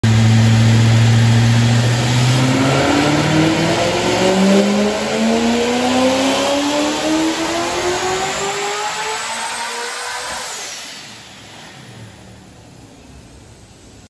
Matt's HPR 427 LS7 Procharged C6Z on the dyno at Davenport Motorsports putting down 1320 rwhp
HorsePower Research built Dart 427 LS7 build with Brodix cylinder heads, solid roller Comp Cam, Crower rockers and more!

Don't mind the smoke at the end, slicks just started to rub the inner fenders.